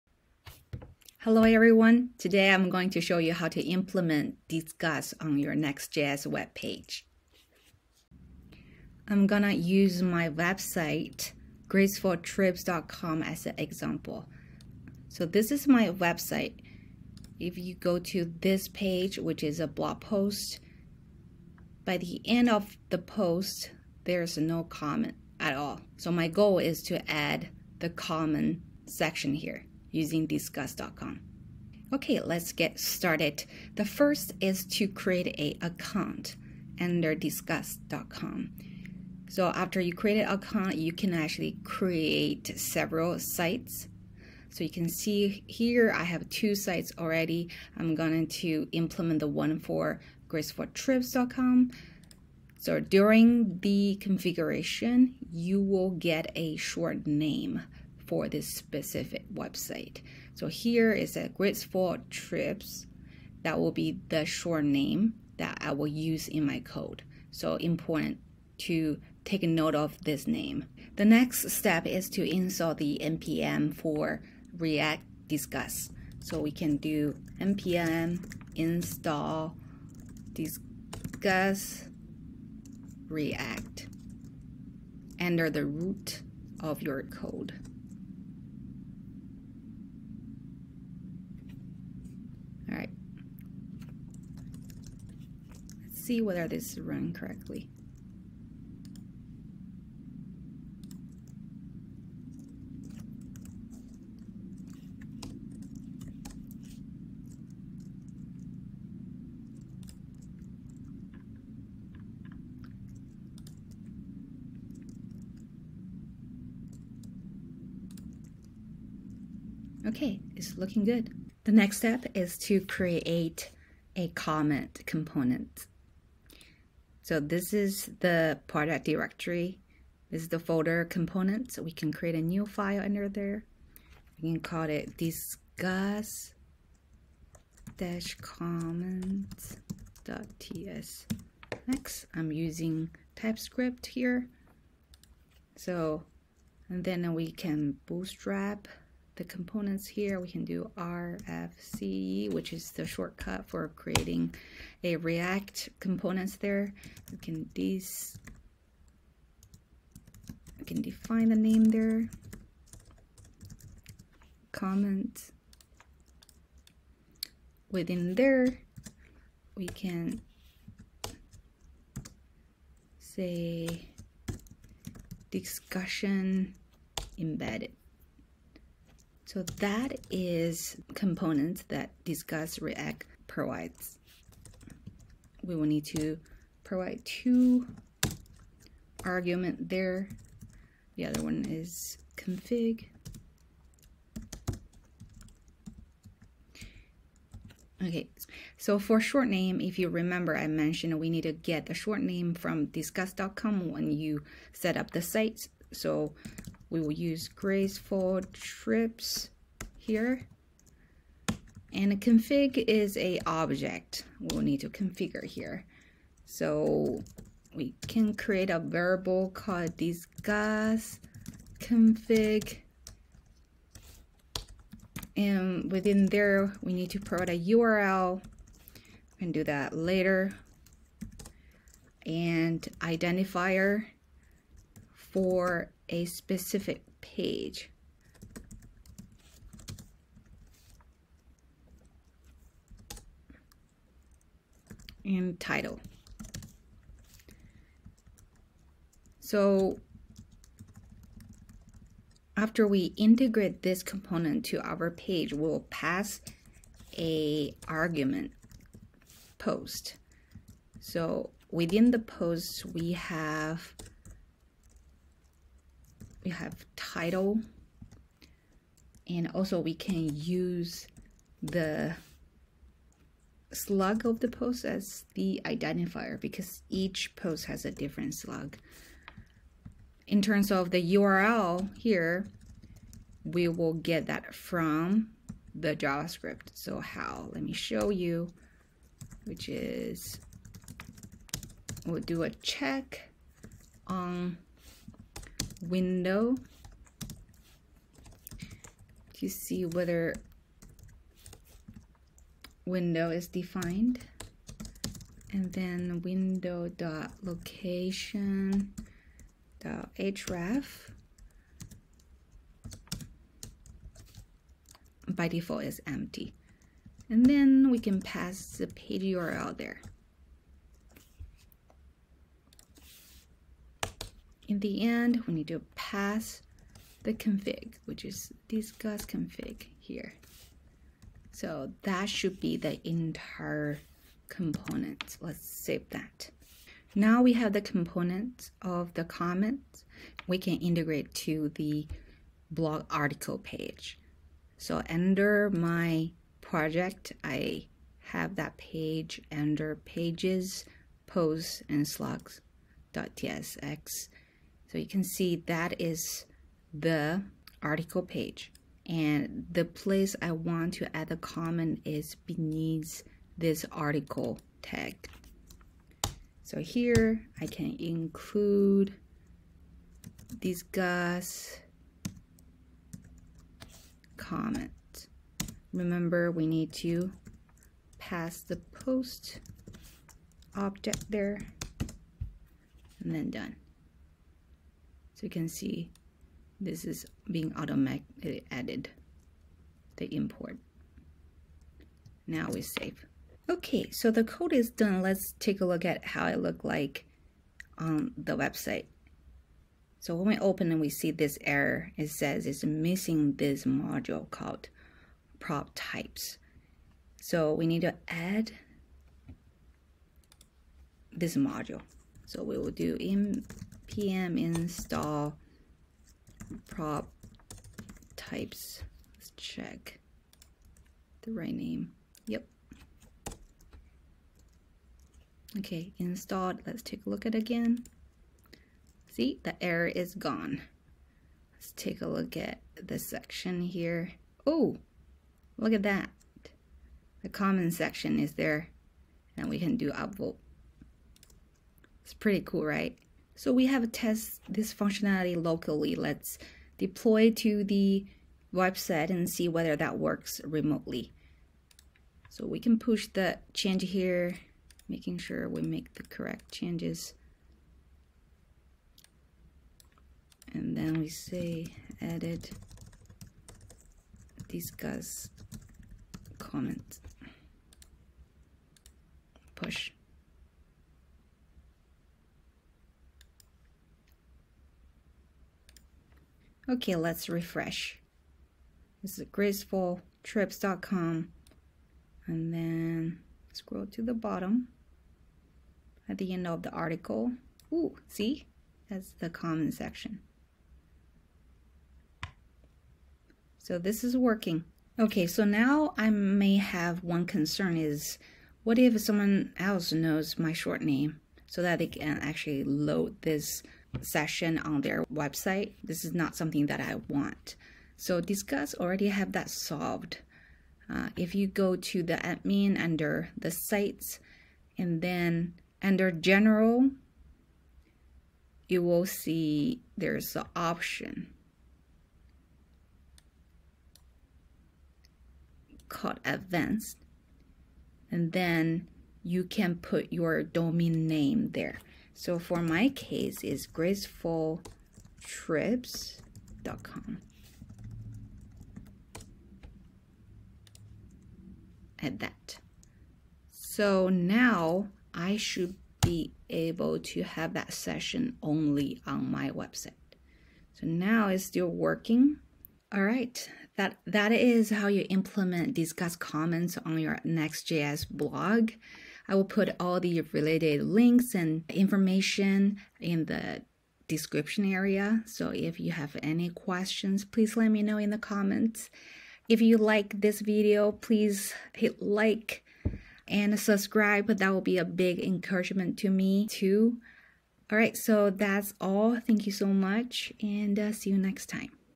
How to Integrate Disqus on your Next.js website
In this video, I will show you step-by-step how to integrate Disqus into your Next.js website.

For your convenience, the component you will need to write is provided below, and you can copy the code

For the blog template I'm using to create my blog,